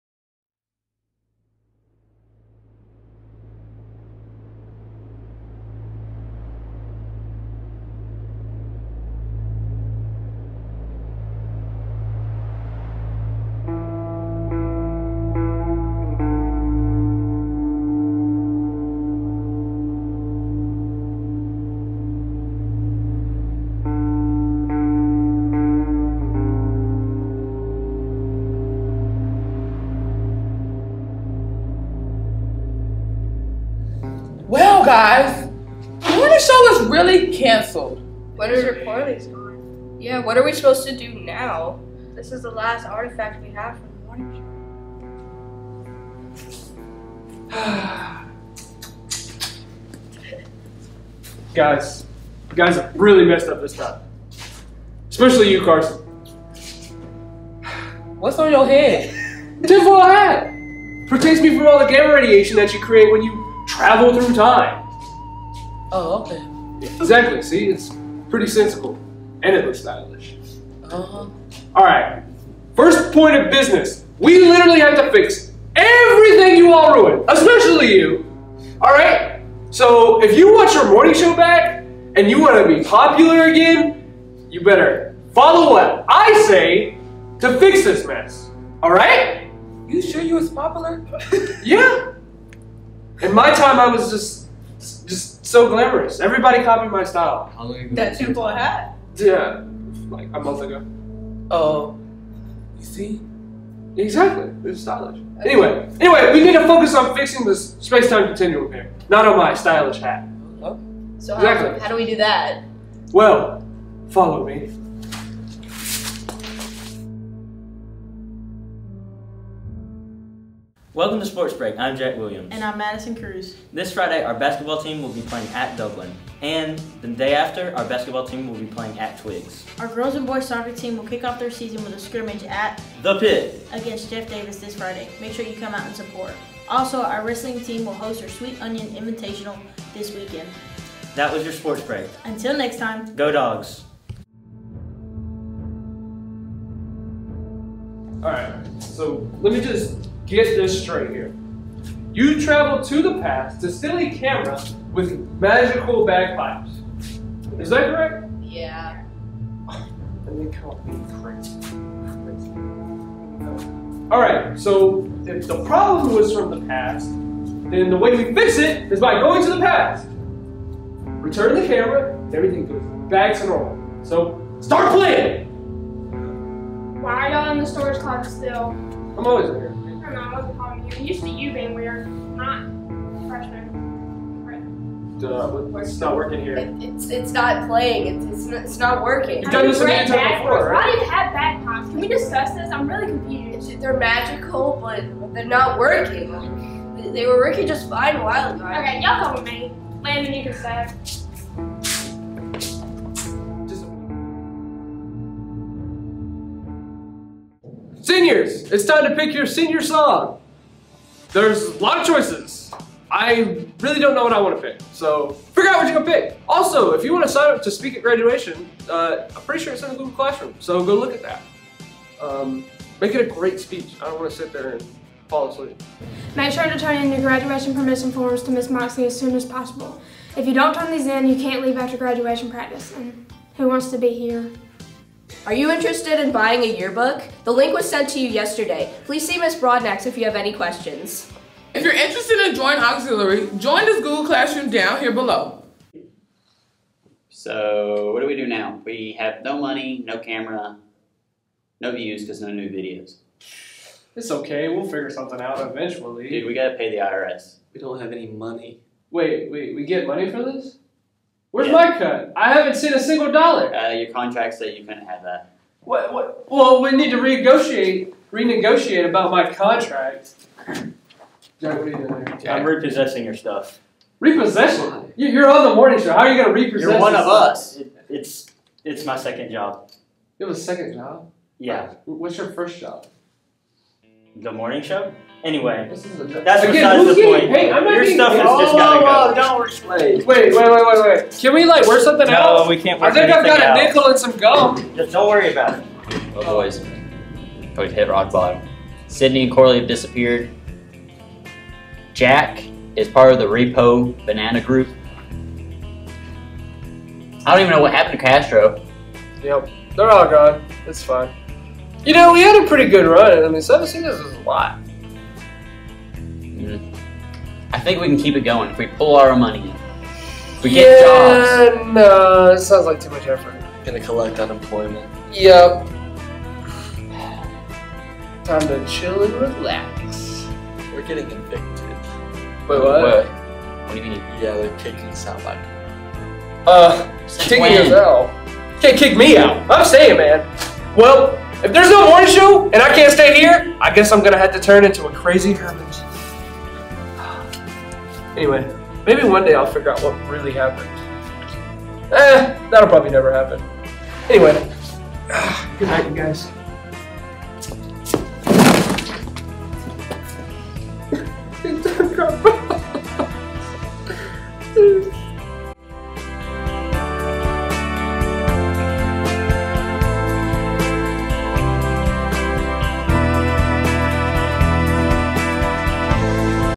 The Morning Show: January 25th, 2024 (S8E4)
Toombs County Schools Morning News for January 25th, 2024